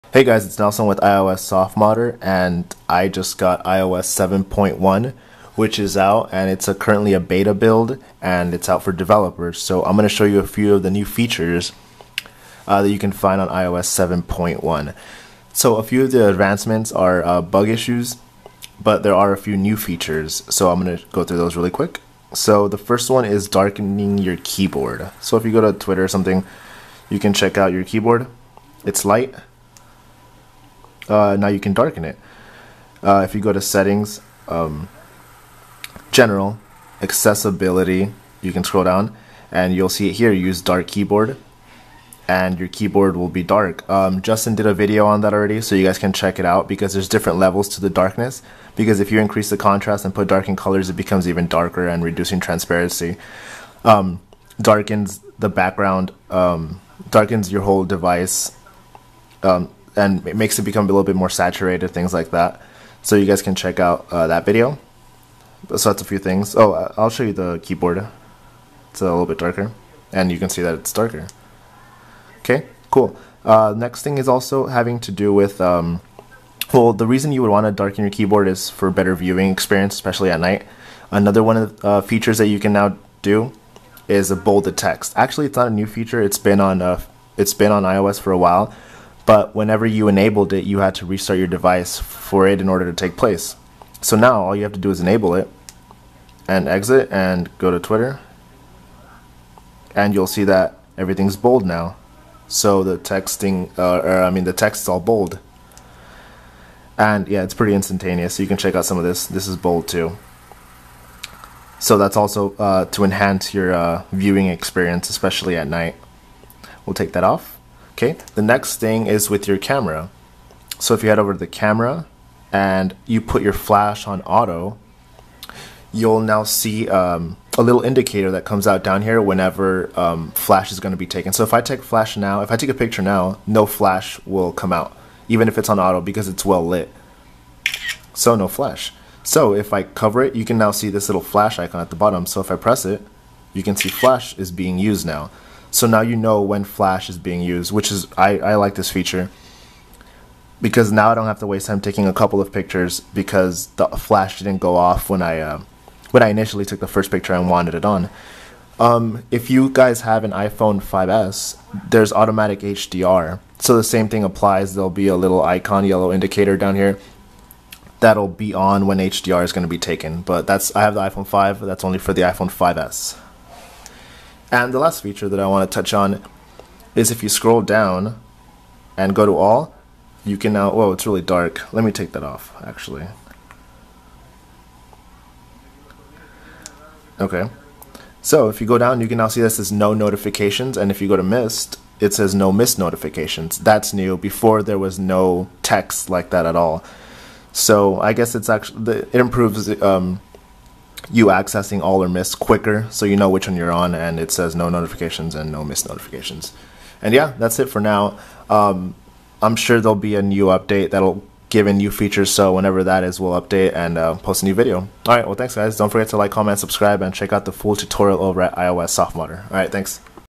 The Cool New Features in iOS 7.1 for iPad, iPhone, & iPod Touch
What's New in Apple's iOS 7.1 Update

In today's softModder guide, I'm going to give you a firsthand look at the new features available in the newly updated iOS 7.1, currently only available in beta for developers.

Also check out out guide on just the dark options in iOS 7.1: